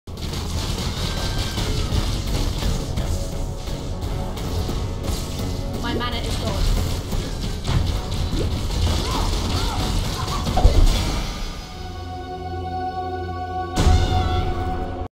Pietits drops Lioneye's number 3
First unique from her in about 50 kills, funnily enough, when I mess up distillate.

Stopped using distillate now and I'm getting uniques again and more rares. Bugged flask?